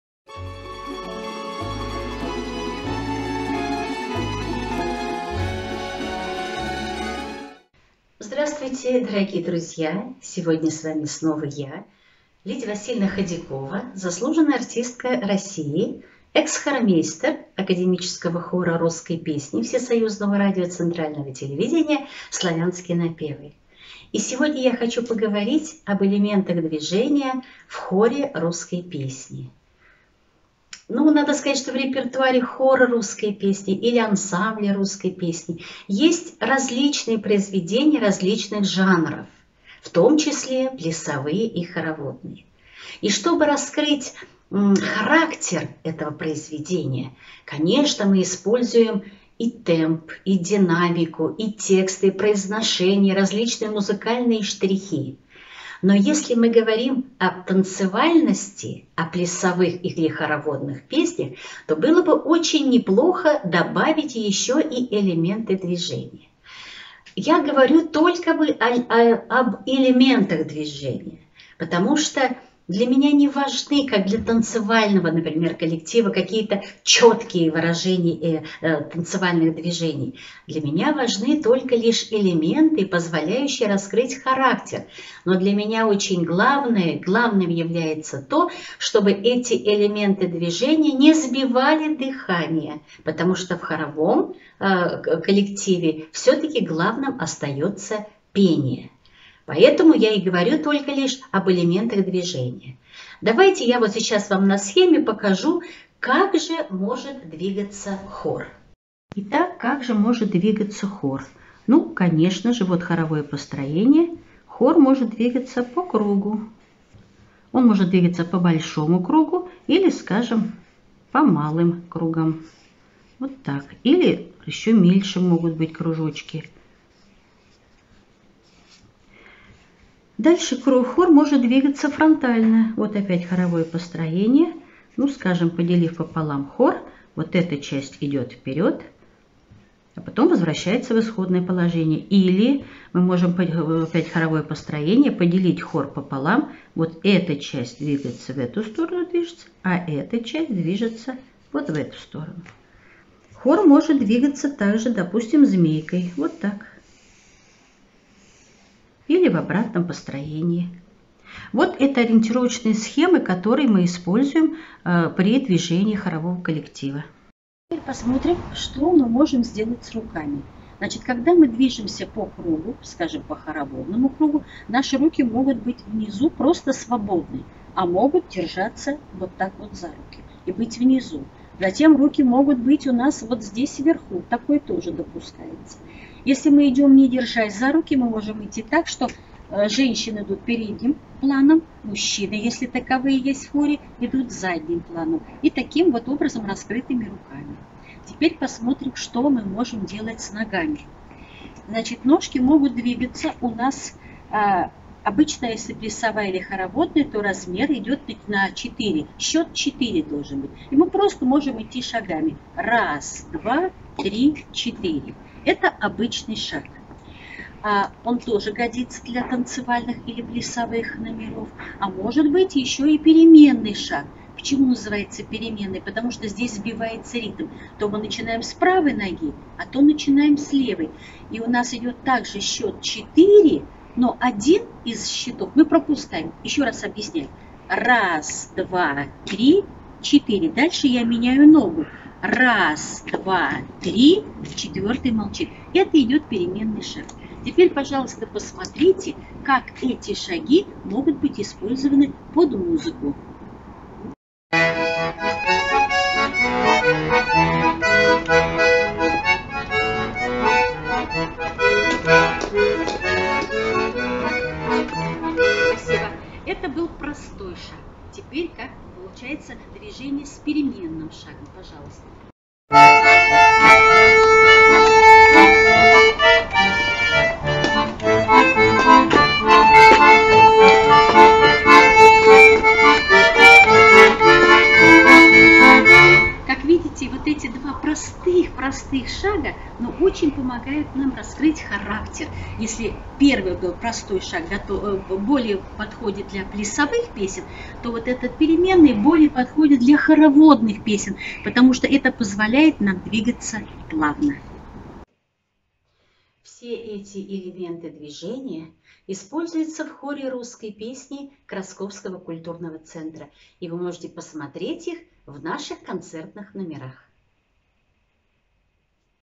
Поговорим о русской песне
Дорогие друзья!

Предлагаем вашему вниманию онлайн-беседу "Поговорим о русской песне" Заслуженная артистка РФ, руководитель народного коллектива "Хор русской песни" Лидия Ходякова расскажет  об элементах движения в хоре русской песни.